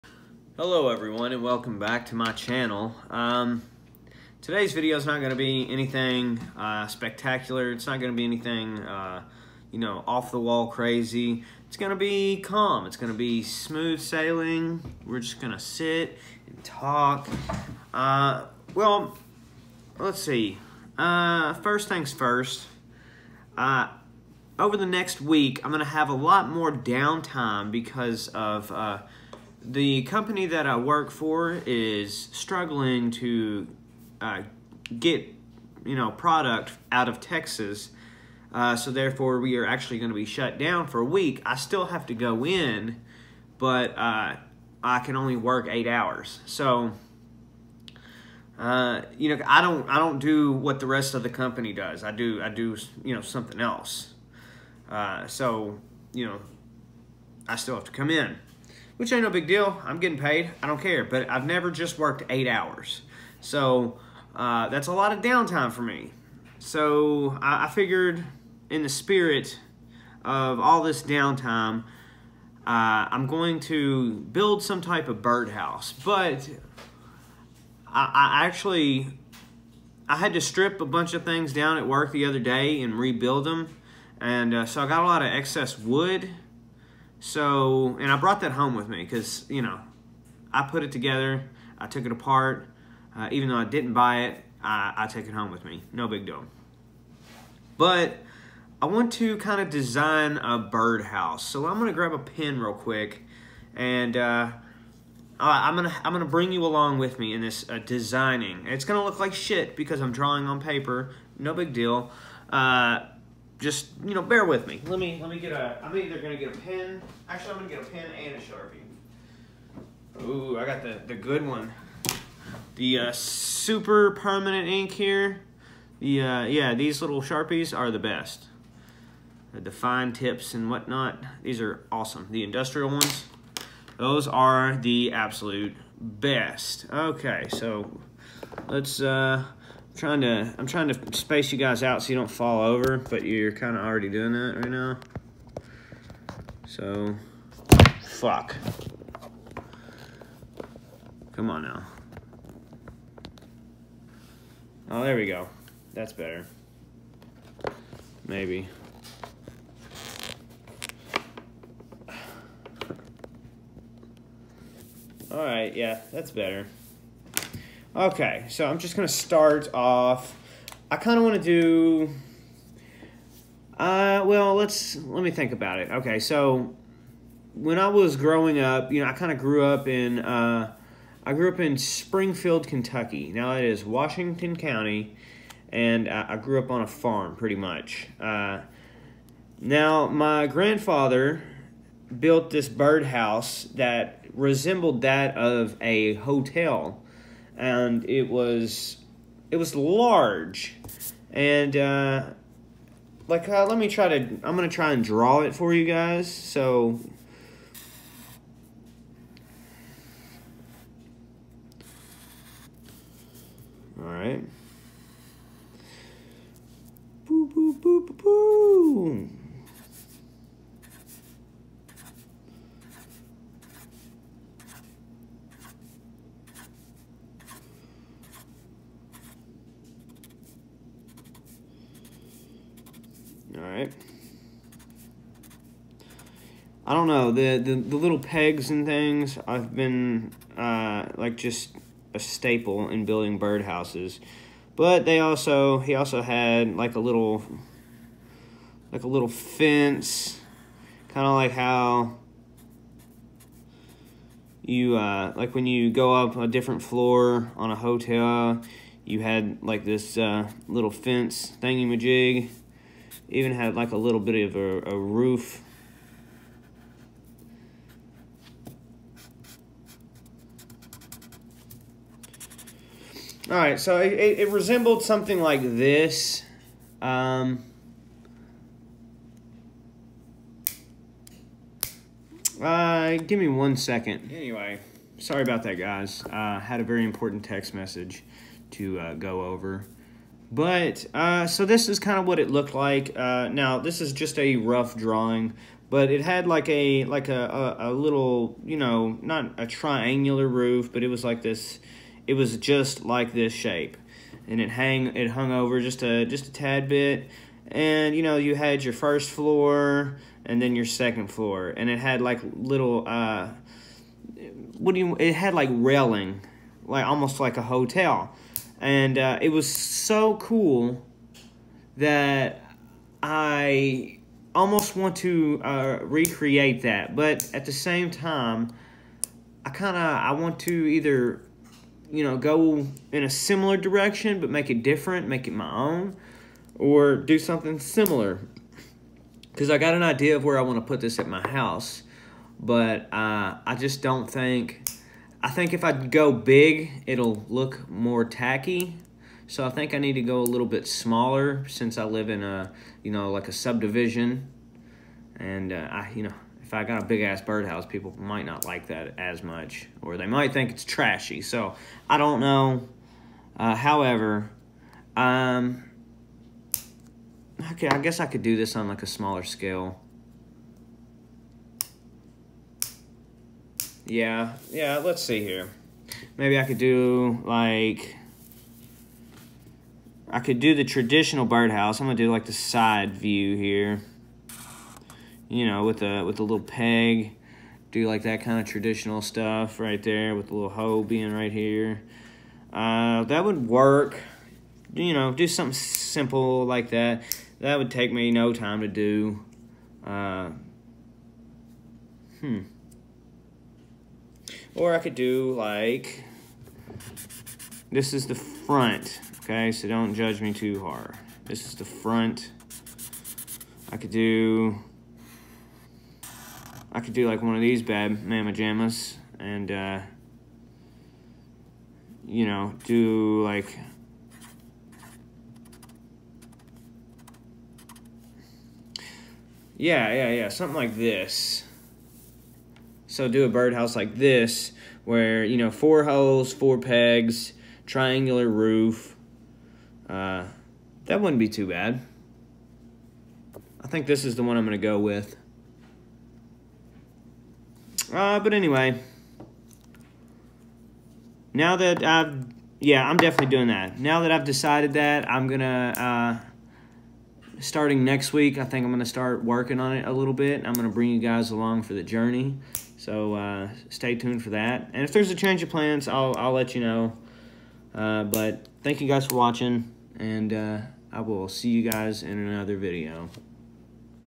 I Might Build A Birdhouse
Tell me which design you think would be the best? I know these are rough drawings, but based on what you see, which one would be the coolest? Have an amazing day!